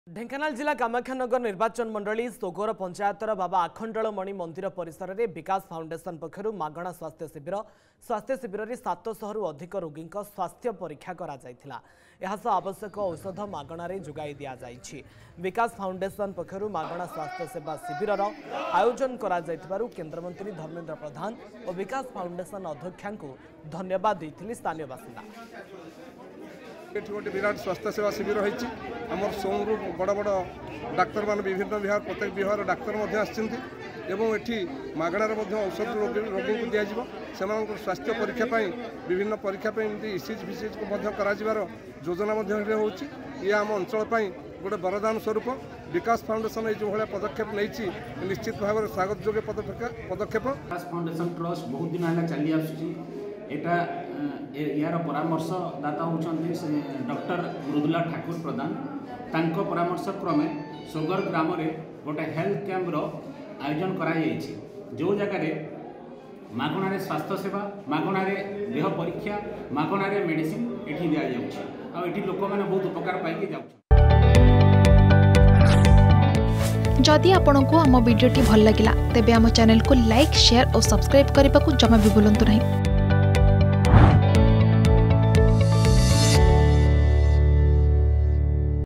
କାମାକ୍ଷାନଗର ସୋଗର ପଞ୍ଚାୟତରେ ମାଗଣା ସ୍ବାସ୍ଥ୍ୟ ଶିବିର || Bikash Foundation
ଢେଙ୍କାନାଳ ଜିଲ୍ଲା କାମାକ୍ଷାନଗର ନିର୍ବାଚନ ମଣ୍ଡଳୀ ସୋଗର ପଞ୍ଚାୟତର ବାବା ଆଖଣ୍ଡଳମଣି ମନ୍ଦିର ପରିସରରେ ବିକାଶ ଫାଉଣ୍ଡେସନ ପକ୍ଷରୁ ମାଗଣା ସ୍ବାସ୍ଥ୍ୟ ଶିବିର ।